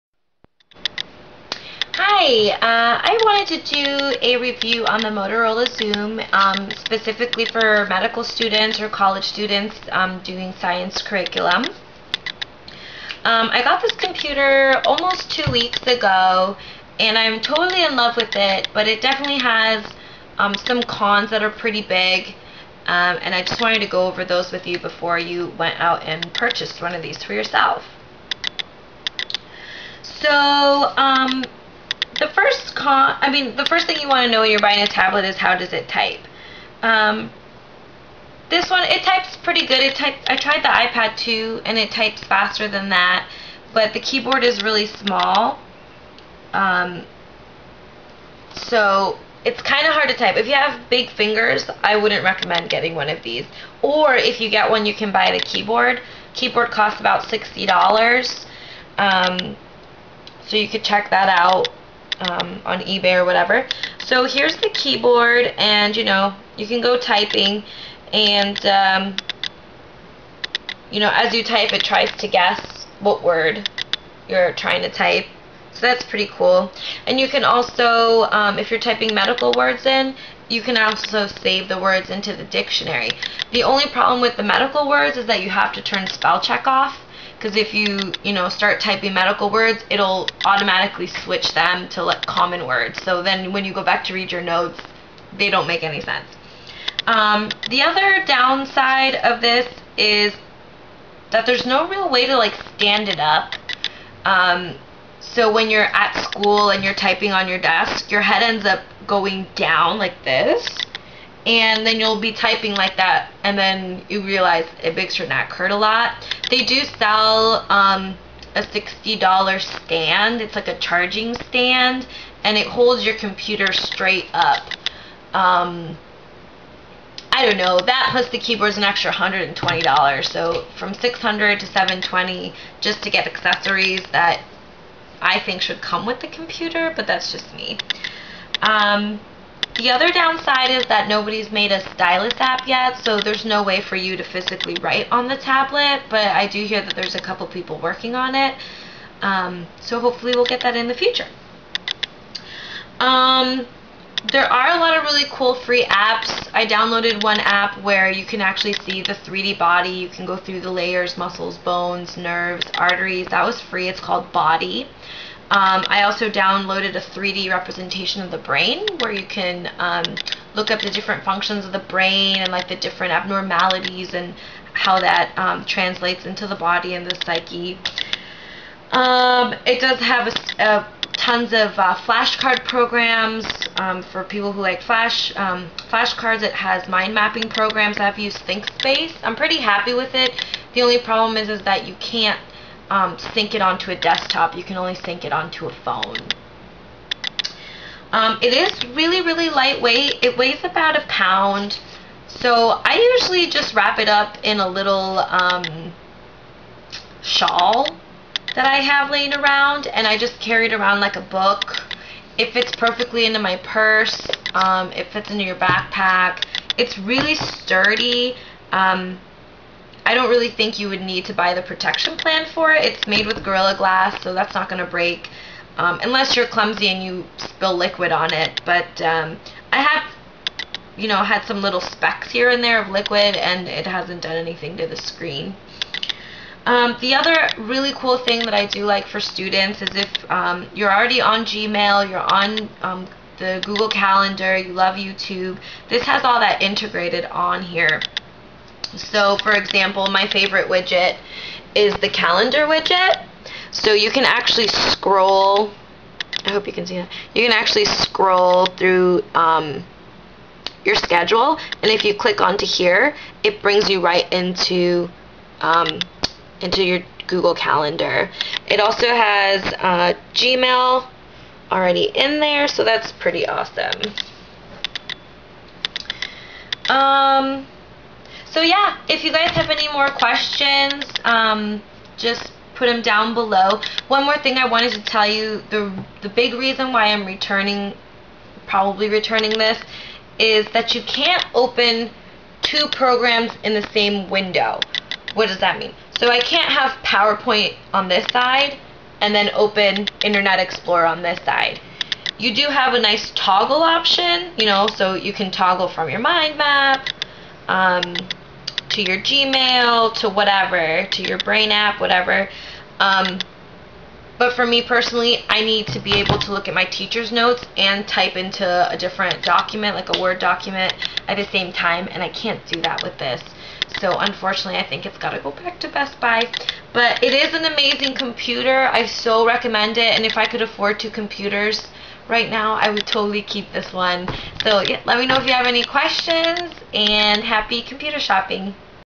Using Xoom Tablet for College or Medical School Use (Review)
Taking a science curriculum? Thinking about buying a tablet?
Check out this video for a list of PROs and CONs